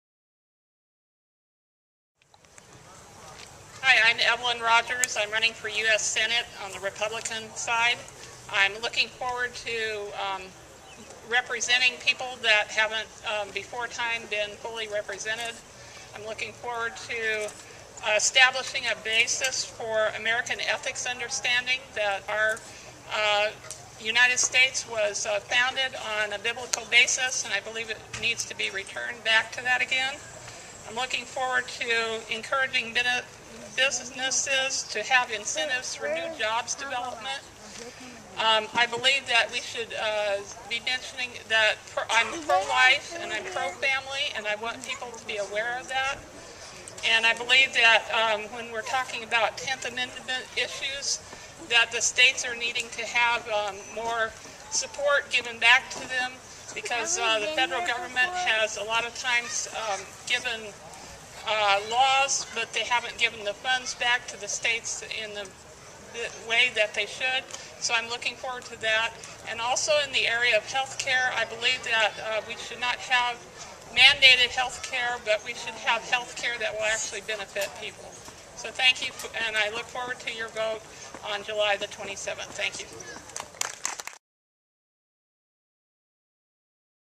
Evelyn Rogers at Muskogee Tea Party Voter Education Rally
U.S. Senate candidate Evelyn Rogers speaking at the Muskogee Tea Party's July 2nd Voter Education Rally.